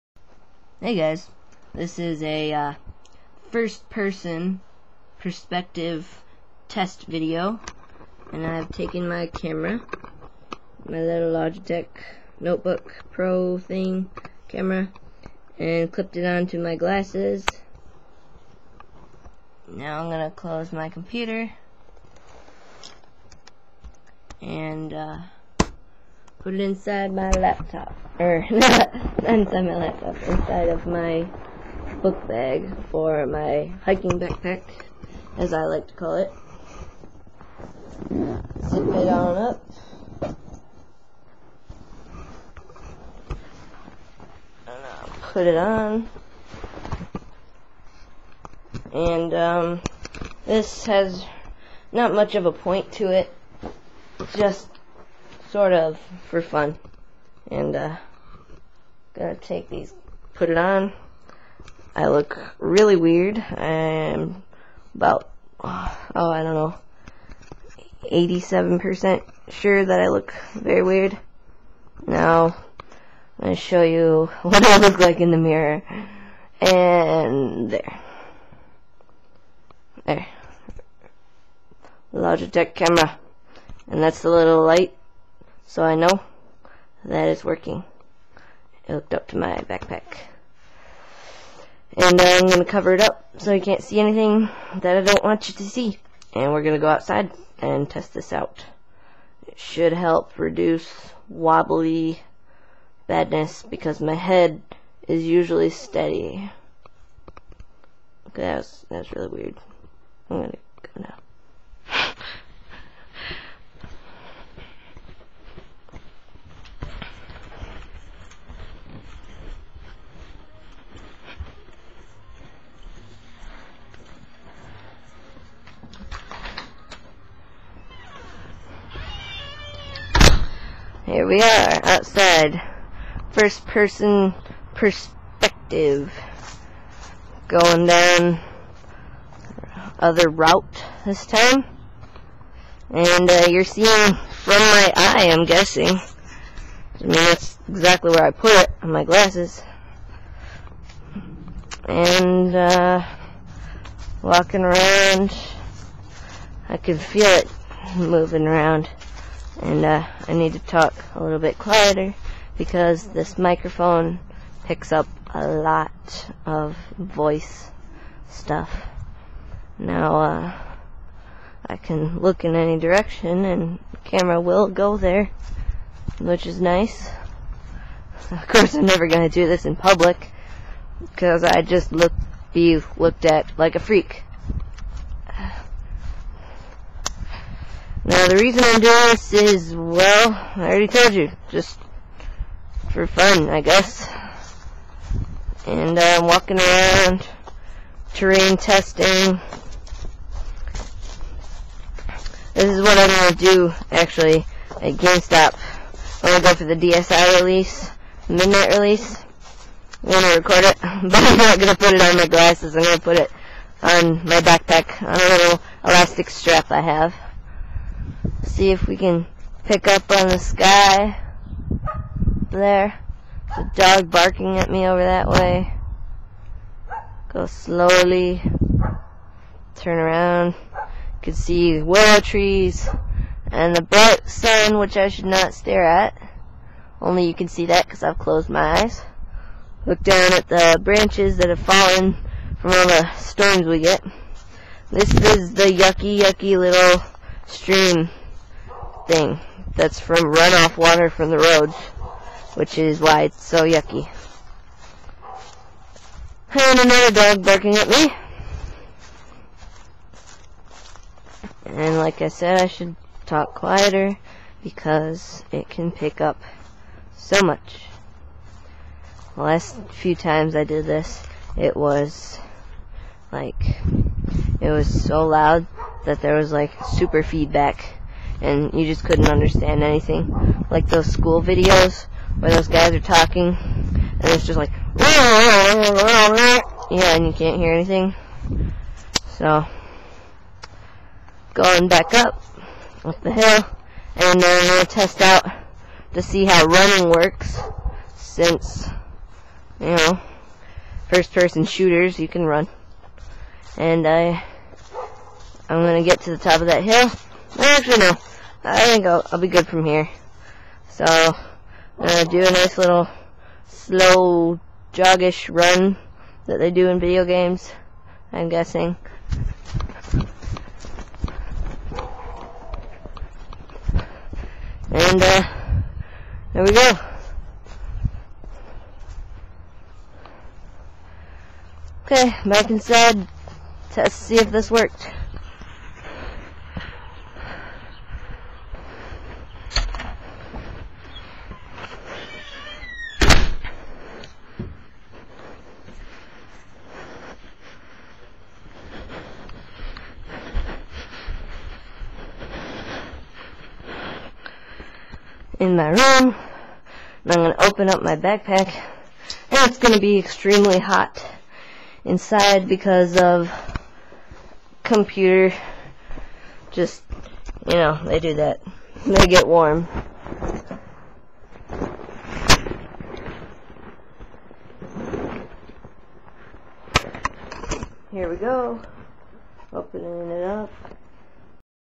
First Person laptop in a bag recording
I use my laptop and webcam to record HD video. This is a test and the sound is fuzzy like in those school videos sometimes. Its only because i talk to loud and my camera mic picks up all the talking it can find.